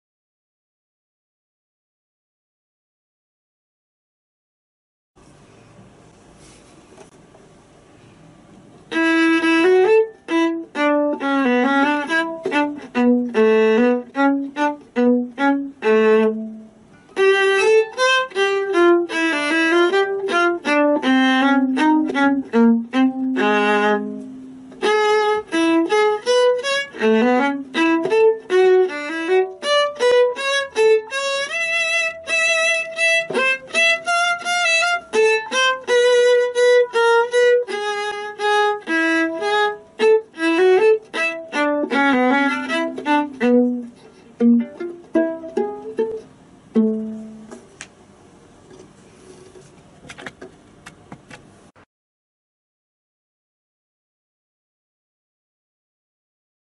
Viola Grade 7 Sight Reading No 1
Grade 7
No. 13 Con brio
Played by Mike Chow